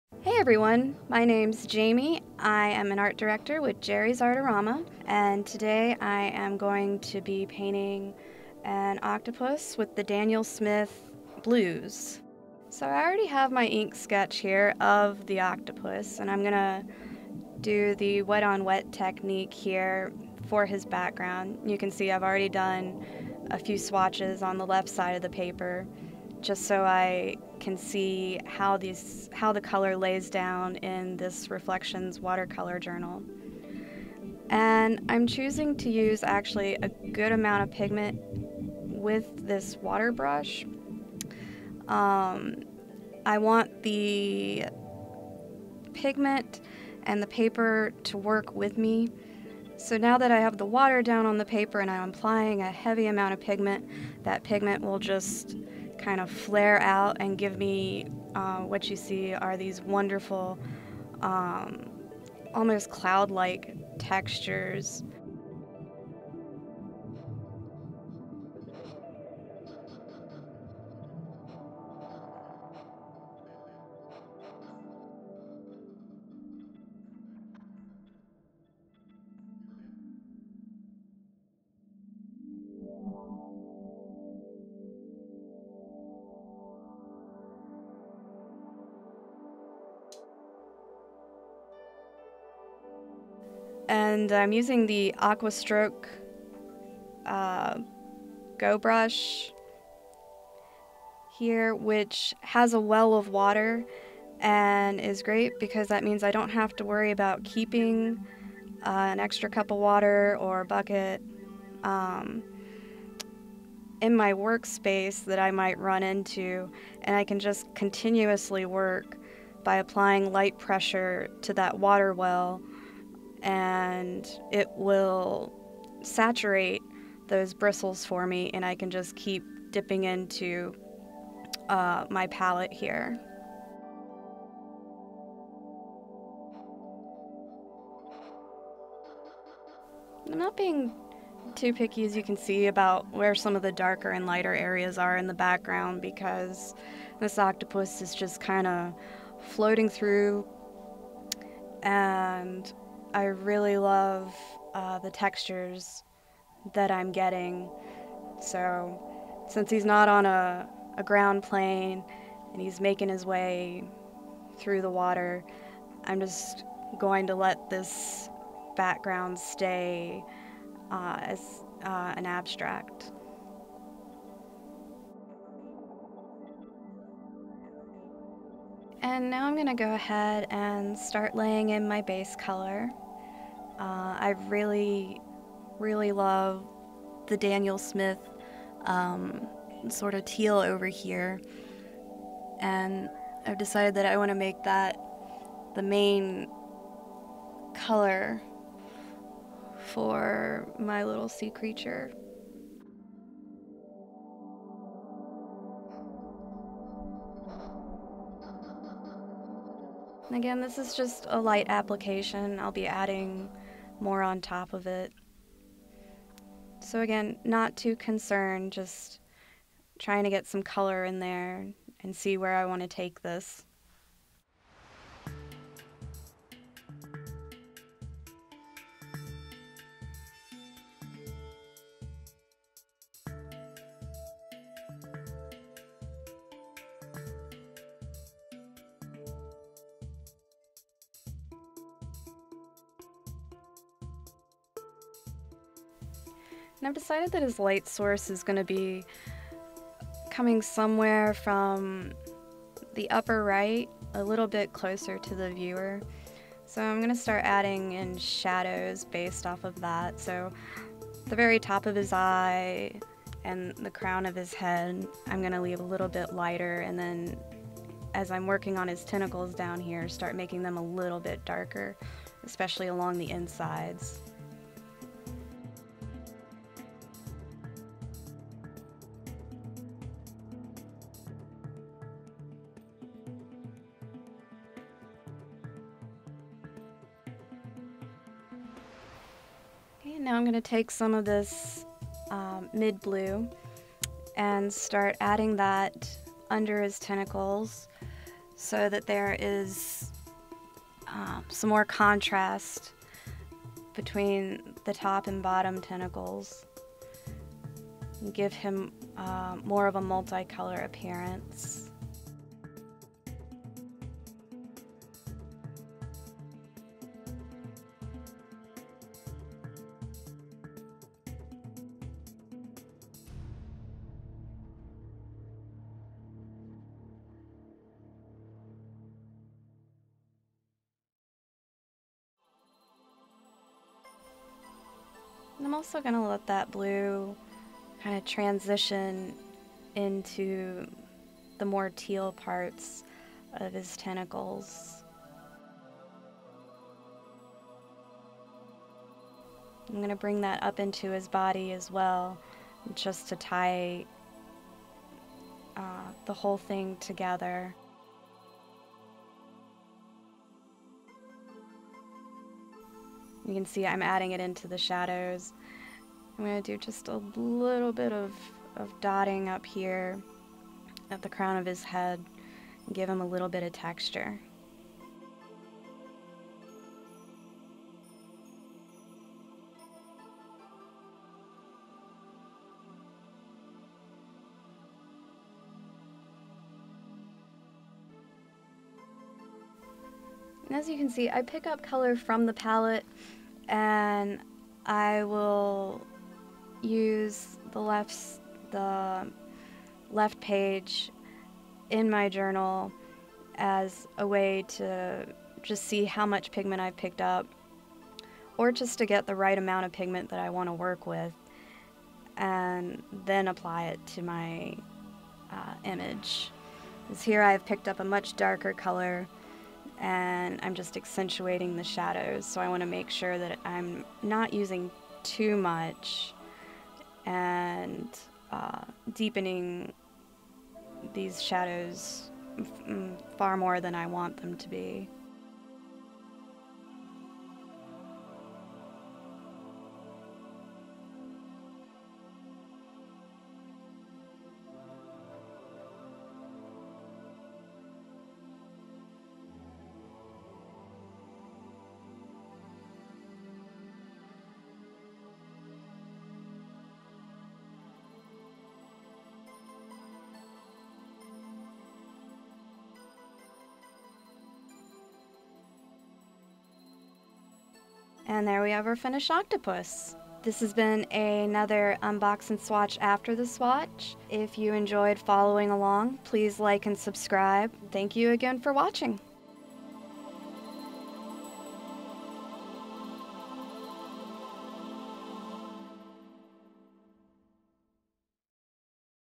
After the Swatch - Painting an Octopus with Daniel Smith Watercolor Blues set of 6
Interested in any of the supplies used in this video?
Daniel Smith Extra Fine Watercolors - Blues: Serene to Dramatic set of 6
Daniel Smith Extra Fine Watercolors - Blues Set with Aquastroke-Go Brush Pens

You've watched the Unbox & Swatch with the Daniel Smith Watercolors Blues: Serene to Dramatic Set of 6, now see the paints in action! Jerry's artist Jaime Fisher paints an octopus using the set while explaining her process and techniques.